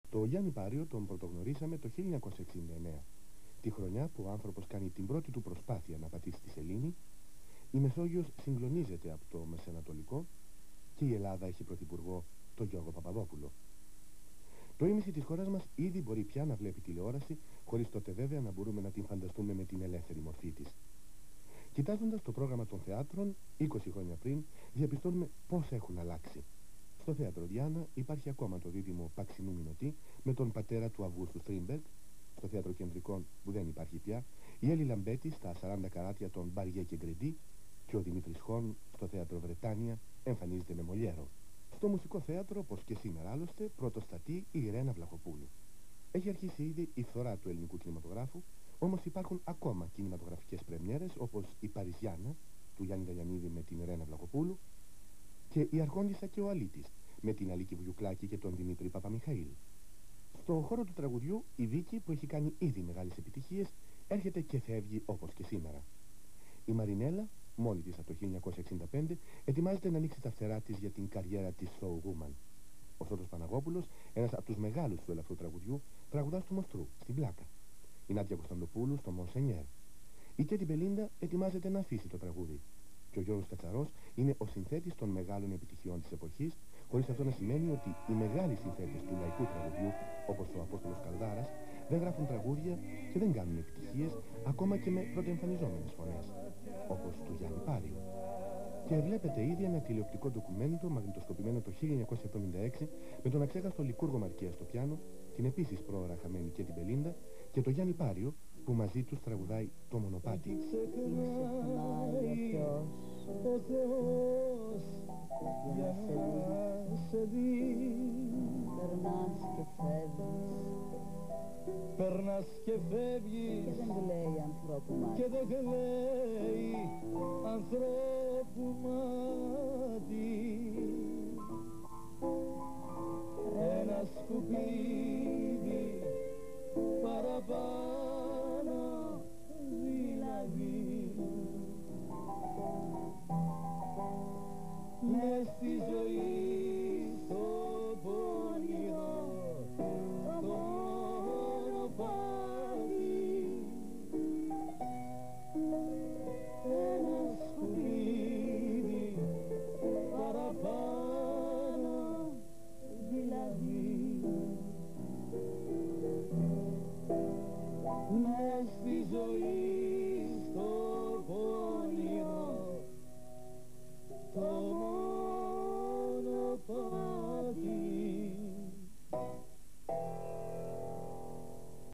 ΓΙΑΝΝΗΣ ΠΑΡΙΟΣ ΑΝΤ 1
Ο Γιάννης Πάριος είναι Έλληνας τραγουδιστής. Θεωρείται ένας από τους δημοφιλέστερους ερμηνευτές των τελευταίων δεκαετιών. Εμφανίστηκε στον χώρο του τραγουδιού το 1969 και έκτοτε έχει συνεργαστεί με πολλούς τραγουδιστές και δημιουργούς.
Μεταδόθηκε 01 Ιανουαρίου 1990 στον ΑΝΤ-1.